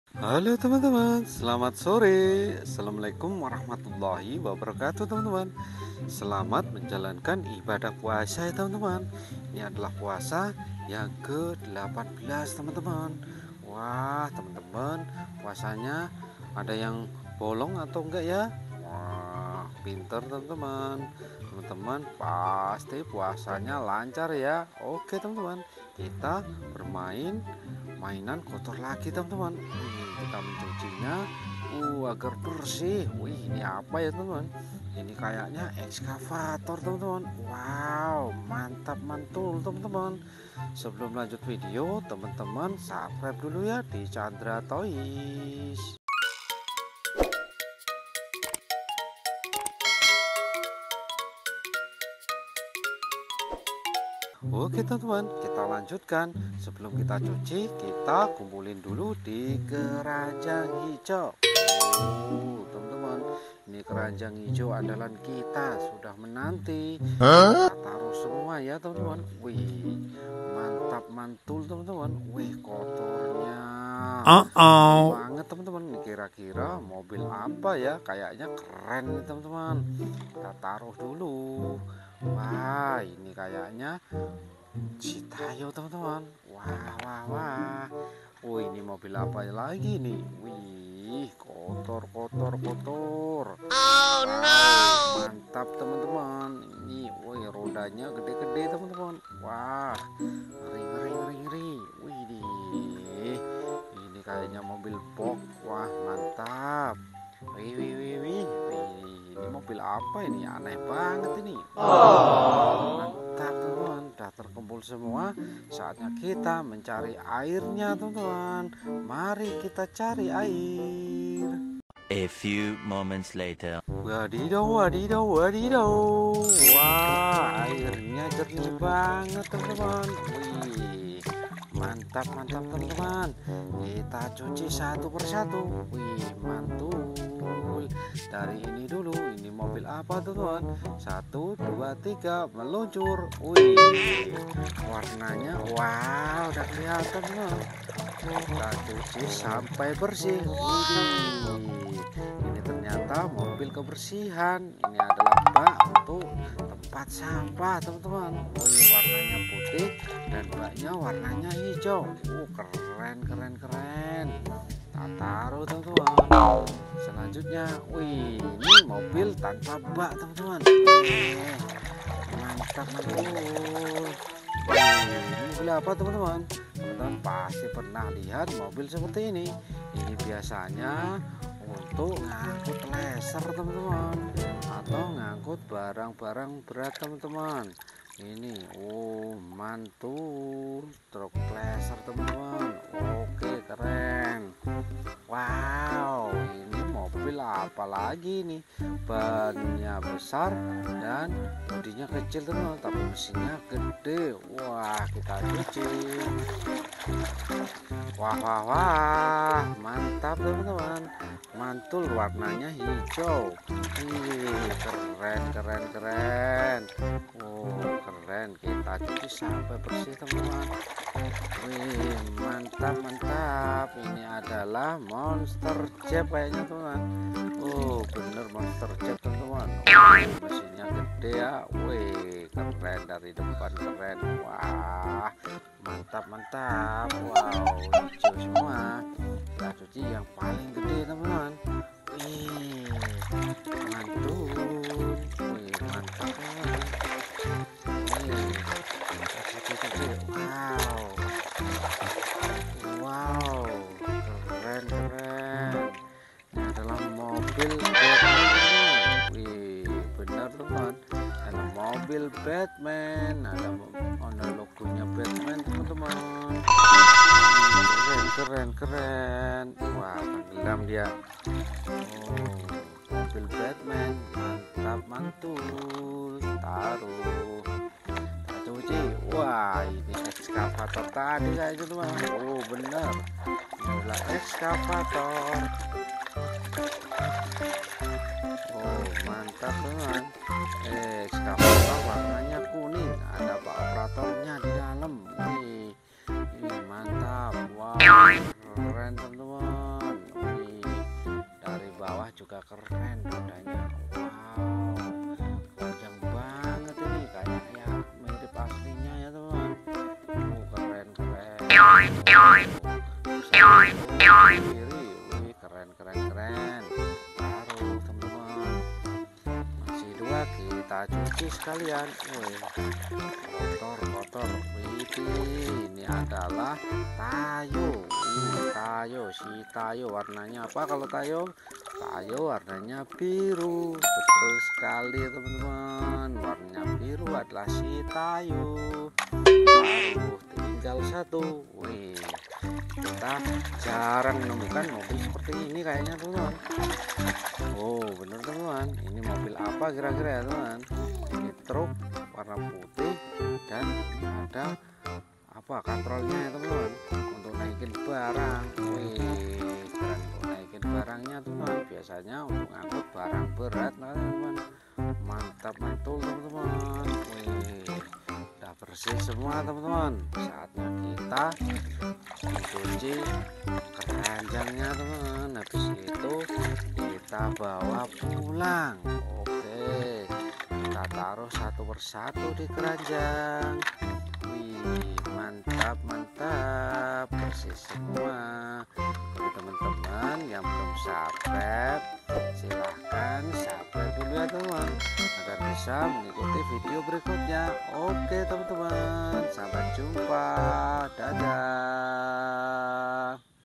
MEMBERSIHKAN MAINAN KOTOR BERLUMPUR | MOBIL BATMAN, JAP MONSTER, EXCAVATOR, TAYO, TRUCK COMBINE
Truck Combine dan Excavator Baru,
Ayo Temen temen kita Membersihkan mainan kotor berlumpur.

Tayang setiap hari pukul 07.00 WIB dan 20.00 WIB
Terima Kasih